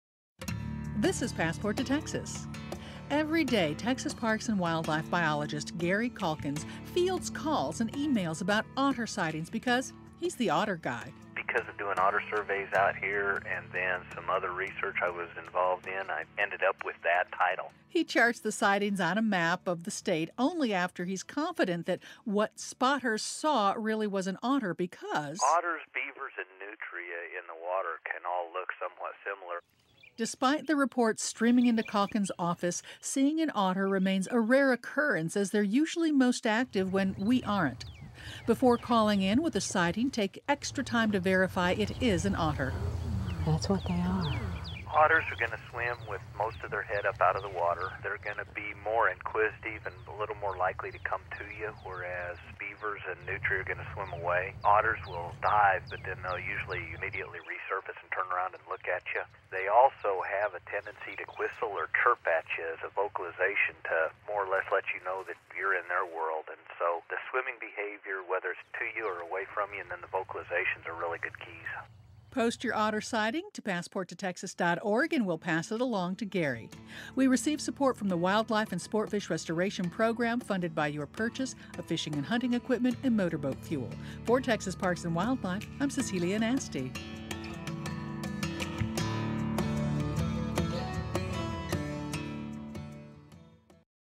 Is That an Otter in the Water?
Seeing a River Otter is a rare occurrence because they are most active when we are not.
They seem to be very playful and inquisitive animals.